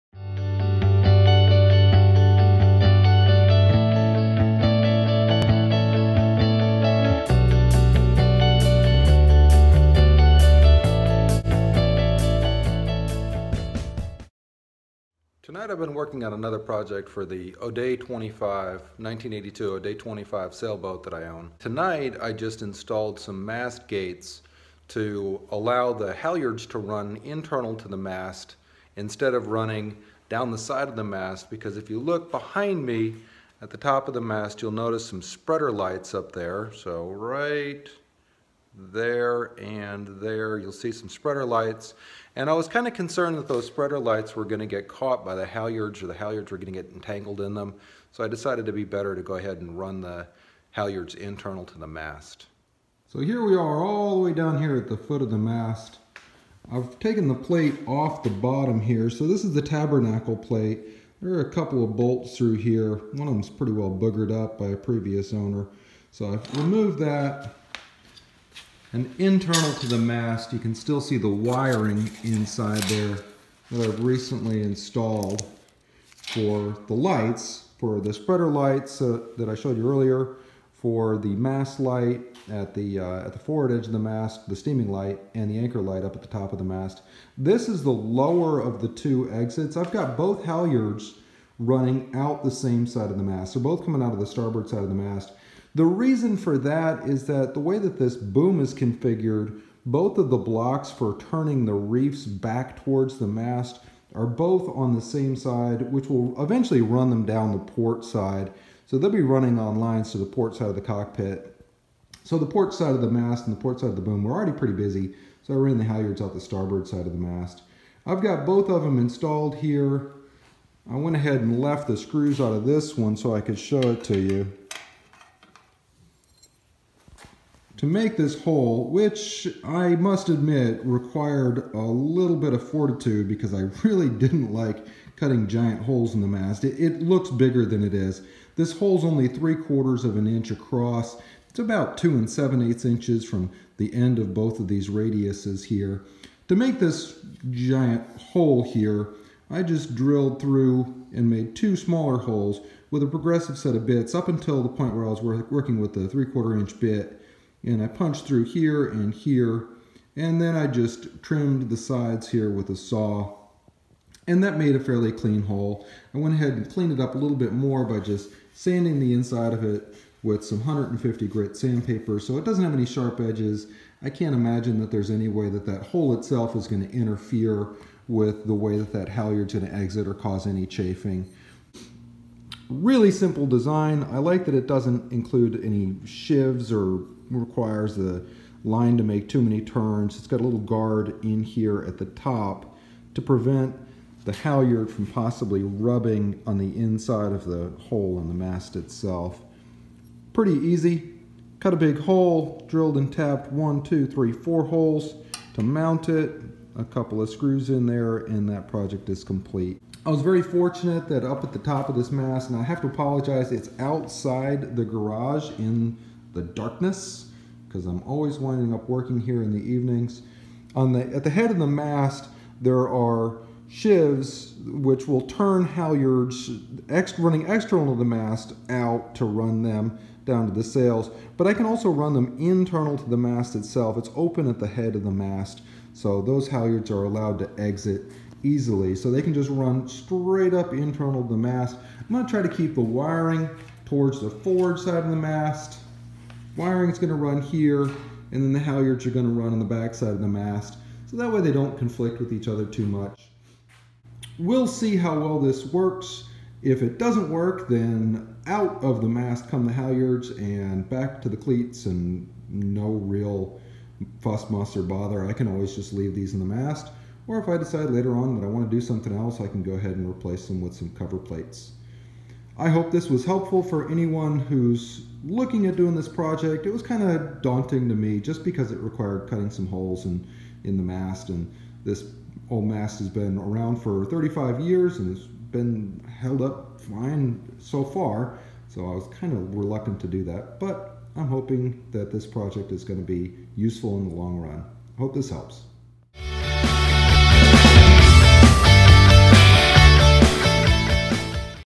Installing halyard exits on a sailboat mast
I installed two halyard exits on the mast of my 1982 O’Day 25.